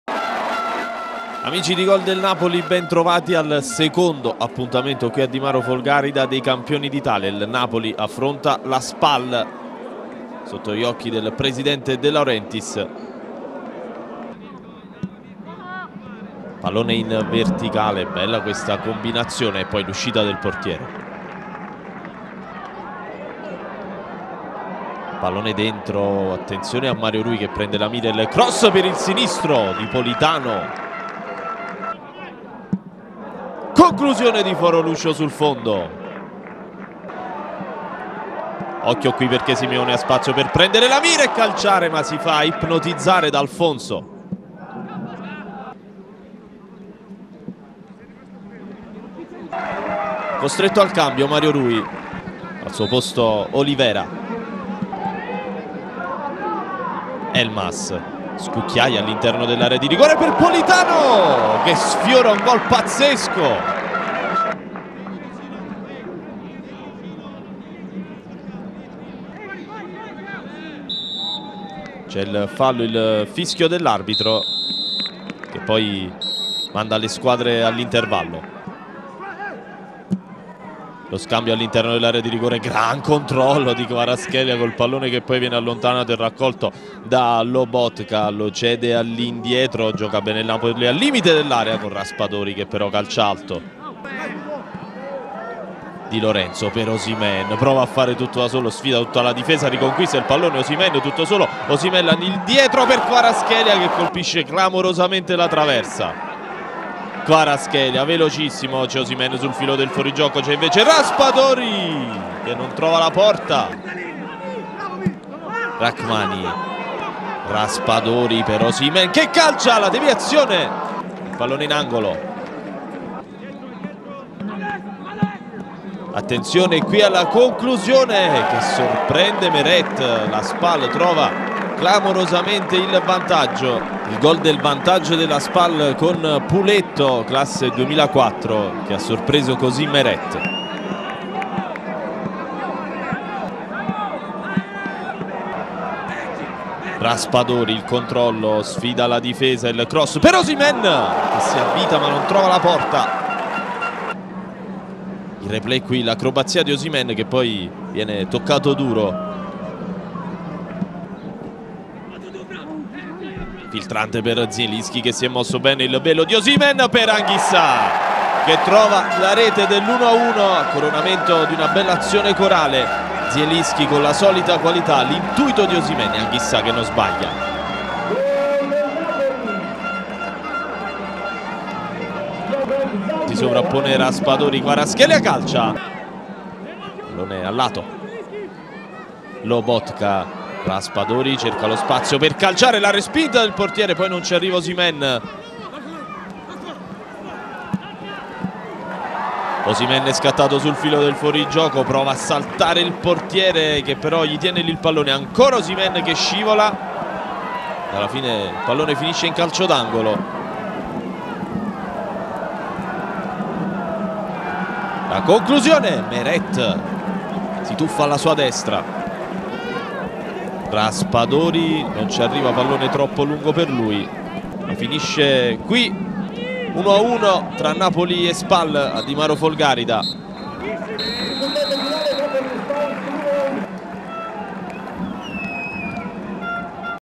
Napoli-Spal 1-1, gli highlights
NAPOLI (4-3-3): Gollini (dal 46’ Meret); Zanoli (dal 46’ Di Lorenzo), Ostigard (dal 46’ Rrahmani), Juan Jesus (dal 46’ Obaretin), Mario Rui (dal 35’ Olivera); Folorunsho (dal 46’ Anguissa), Demme (dal 46’ Lobotka), Elmas (dal 46’ Zielinski); Politano (dal 46’ Raspadori), Simeone (dal 46’ Osimhen), Zedadka (dal 46’ Kvaratskhelia). A disp.: Contini, Zerbin, Coli Saco. All. Garcia

SPAL (4-3-3): Alfonso (dal 46’ Galeotti, dal 76’ Meneghetti); Dickmann, Peda (dal 60’ Breit), Arena (dal 60’ Dumbravanu), Tripaldelli (dal 60’ Saiani); Parravicini (dal 60’ Boccia), Murgia Celia (dal 46’ Maistro); Orfei (dal 60’ Puletto, Antenucci (dal 60’ Rabbi), D’Orazio (dal 60’ Rao). A disp.: Galeotti, Meneghetti, Rao, La Mantia. All. Di Carlo

Arbitro: Paride Tremolada

Assistenti: Mauro Galetto e Davide Moro

Marcatori: dal 62’ Puletto (Sp), dal 73’ Anguissa (Na)